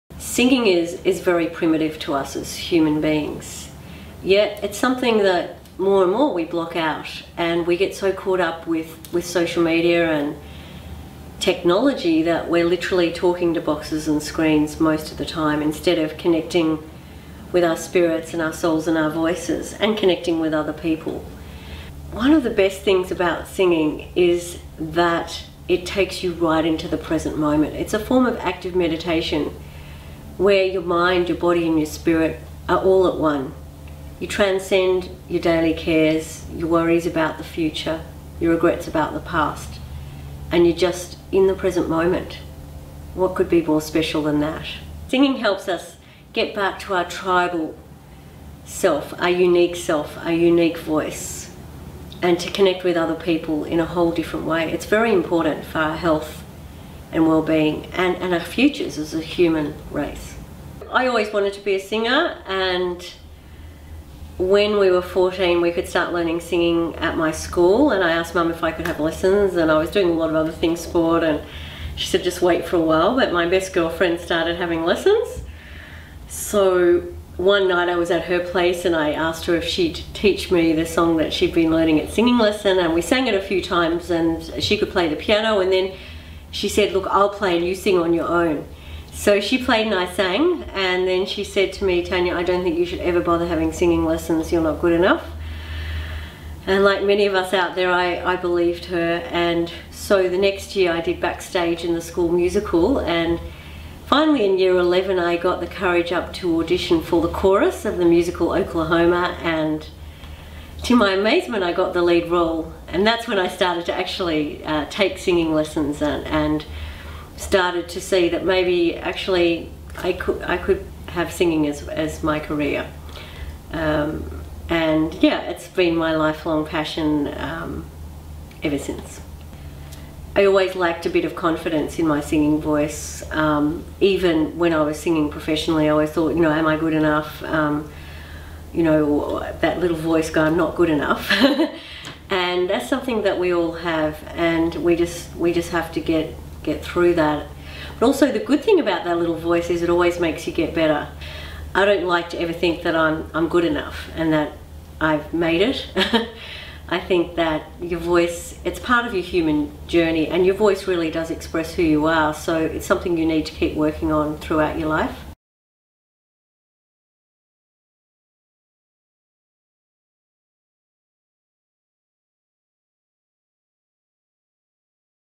Singing, Spirituality and Finding Your Unique Voice - Tania de Jong AM
Tania de Jong AM is Founder and Chair of Creativity Australia's With One Voice choir program. or enter Sing for Spring, Australia's national community singing project,